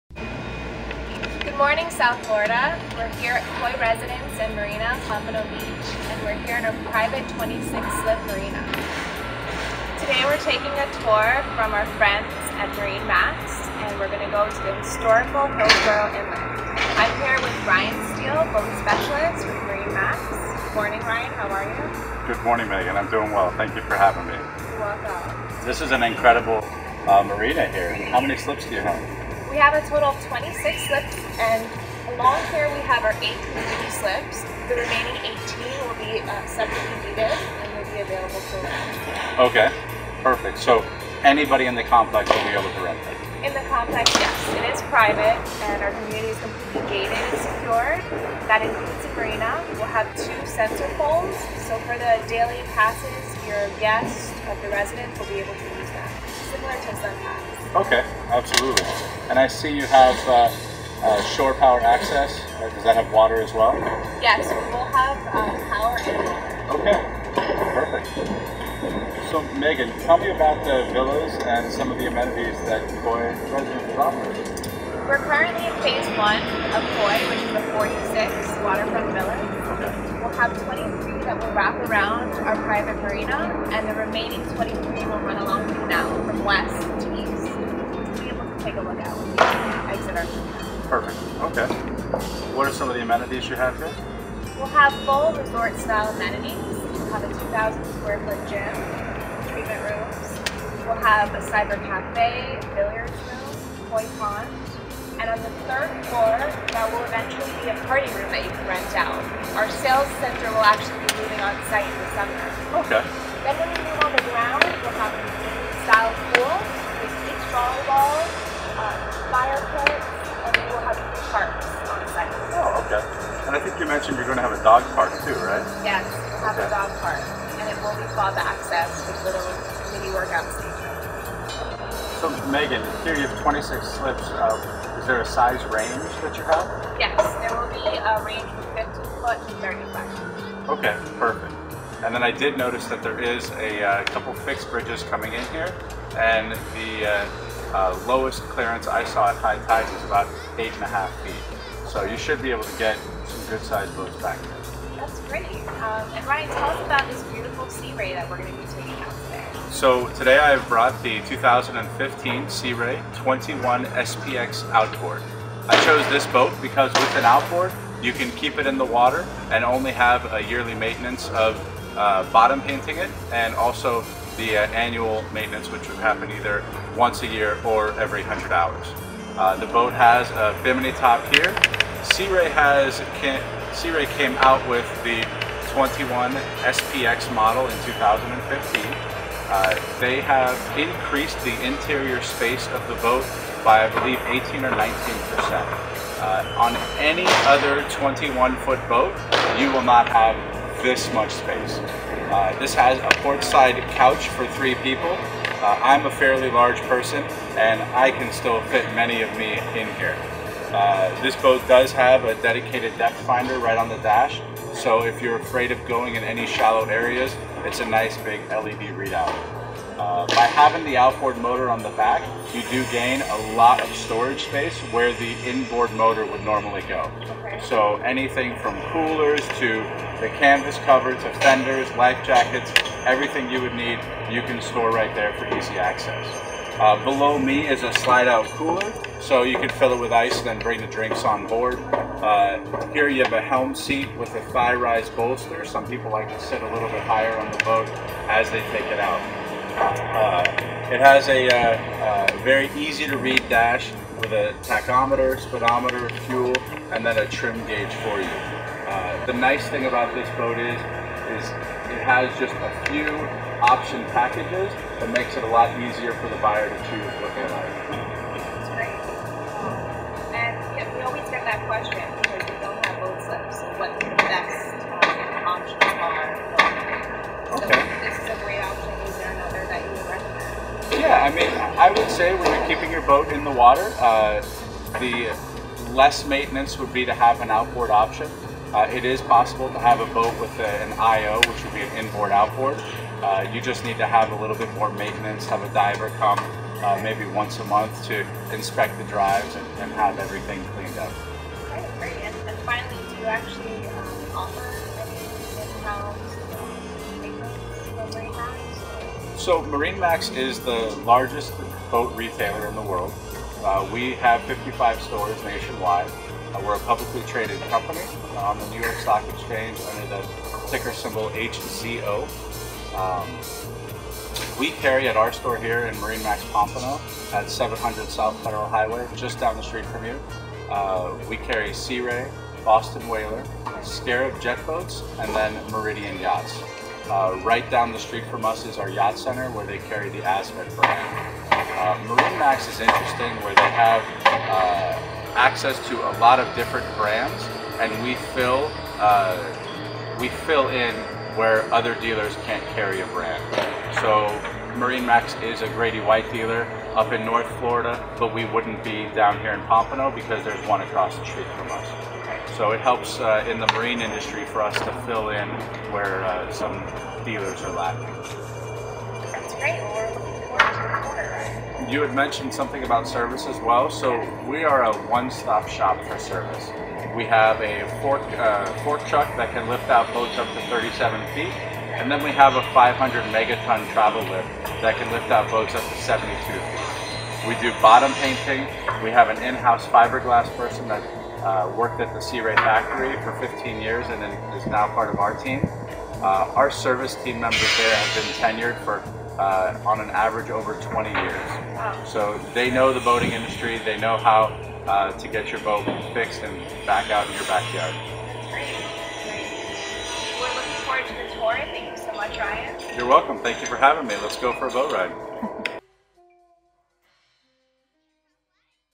Koi Residences & Marina + Marine Max of Pompano Boat tour
Preview a mini boat tour with Koi Residences & Marina + Marine Max of Pompano as we discuss Koi's luxury lifestyle and all things water.